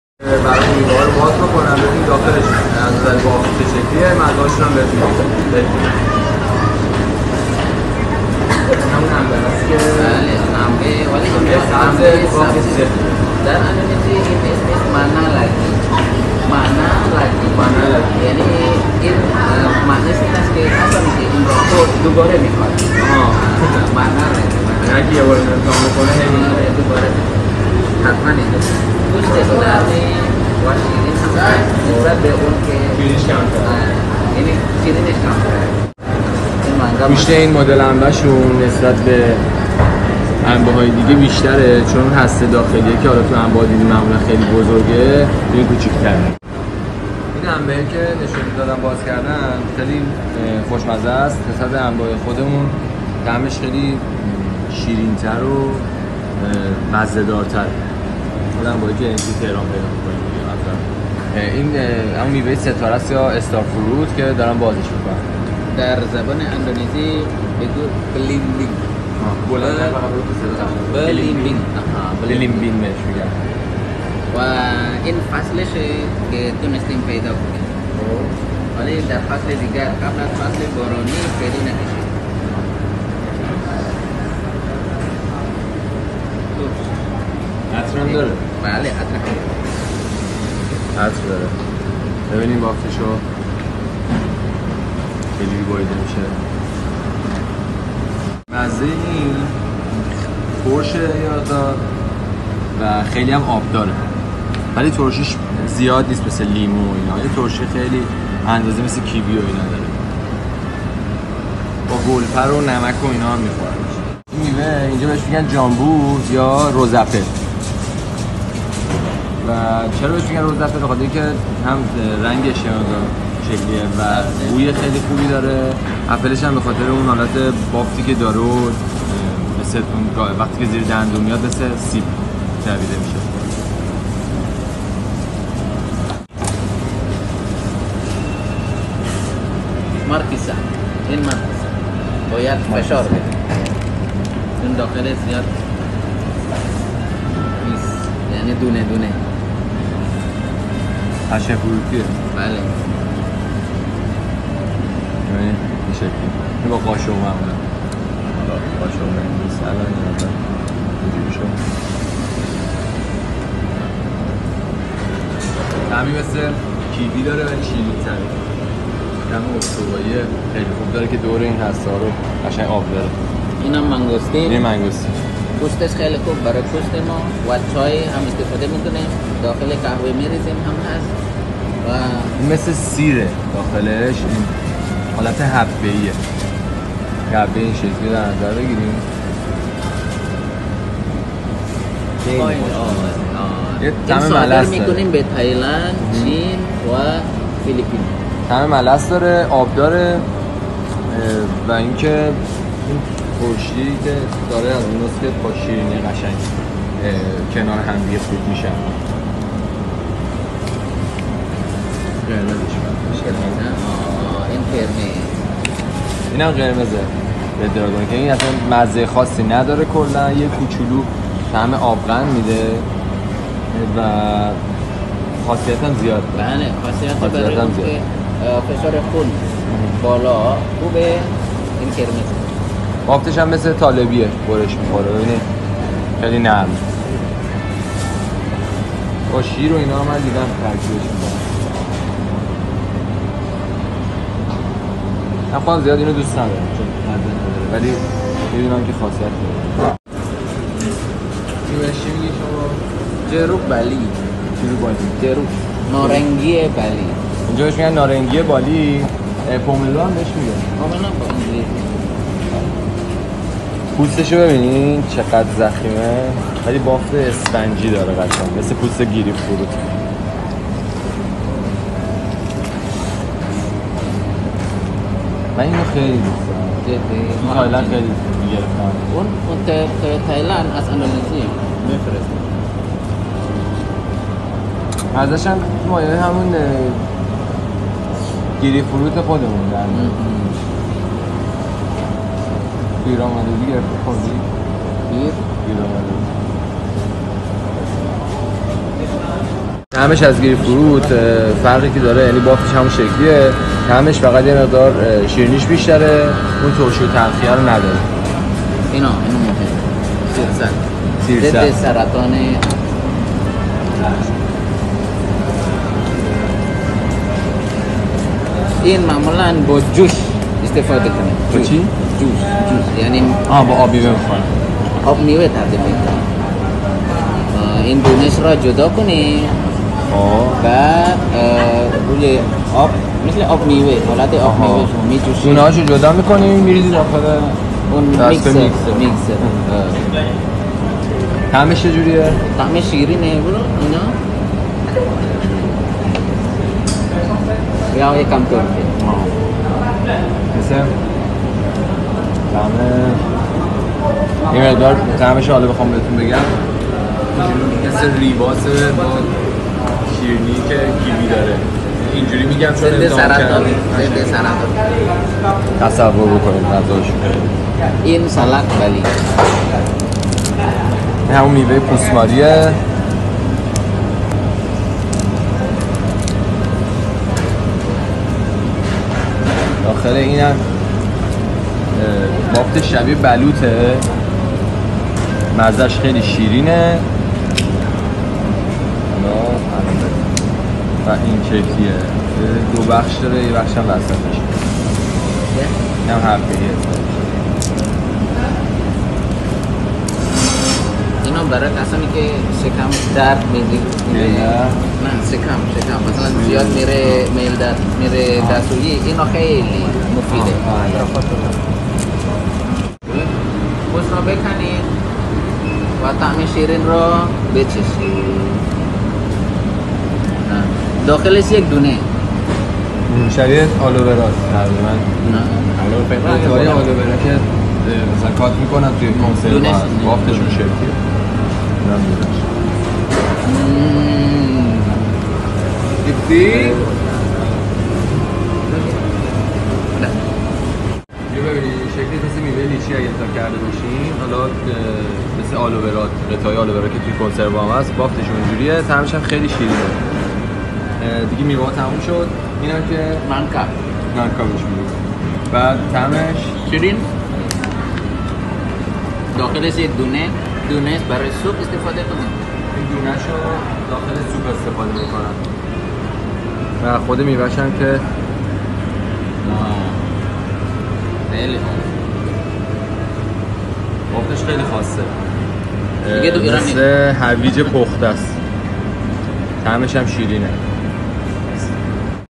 اینم از فیلمِ باز شُدنِ میوه هایِ اُستوایی و مزّه هاشون😍
زَکی تو گرفتن این فیلما خیلی کمک کرد بهم و جا داره ازش تشکّر کنم به خاطر انرژی و معرفتی که به خرج داد...🙌🏽❤️
دو سال ایران زندگی کرده و زبونِ فارسی رو خیلی جالب و خوب یاد گرفته